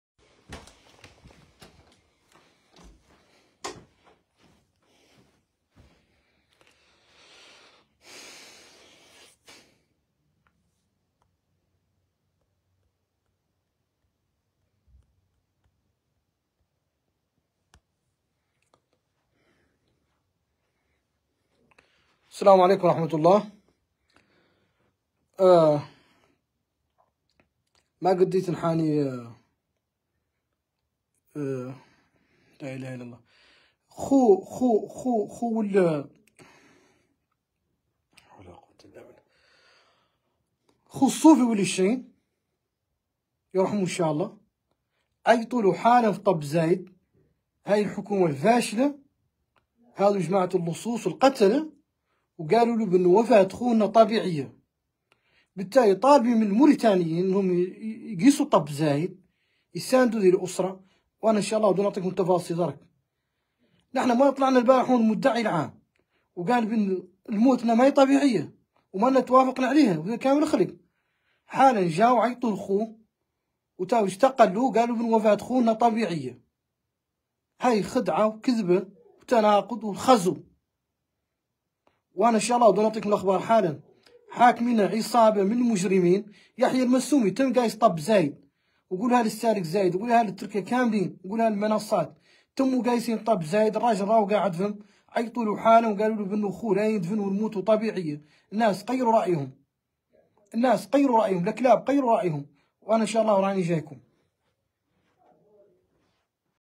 أستدعاء شقيق الصوفي ولد الشين واخباره أن وفات شقيقه تمت في ظروف طبيعة بمعنى قضاء وقدرهذه مهزلة وضحك
تم أستدعاء شقيق الصوفي ولد الشين واخباره أن وفات شقيقه تمت في ظروف طبيعة بمعنى قضاء وقدر.
هذه مهزلة وضحك على الذقون واحتقار لعقول الشعب الموريتاني، نطلب من الجميع النزول للشارع.